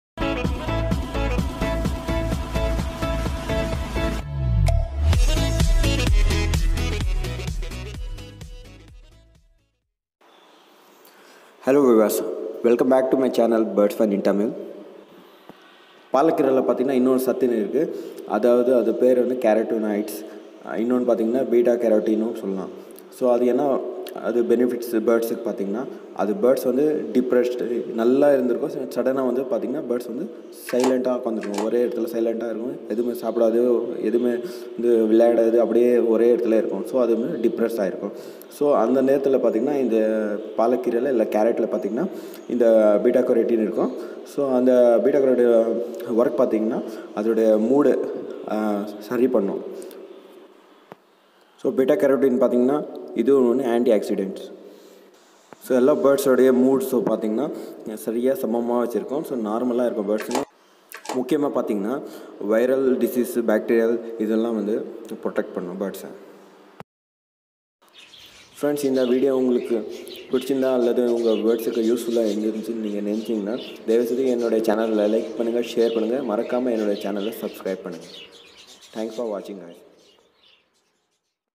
Why spinach important |BIRDS MOOD சரியில்லையா இதை கொடுங்கள்
Hello viewers...Today's our topic is bita carotene and benefits for birds and what it's work for birds .

Video Details.
1.importants of Spinach
2.how to use Spinach
3.what is bita carotine
4.birds mood off problem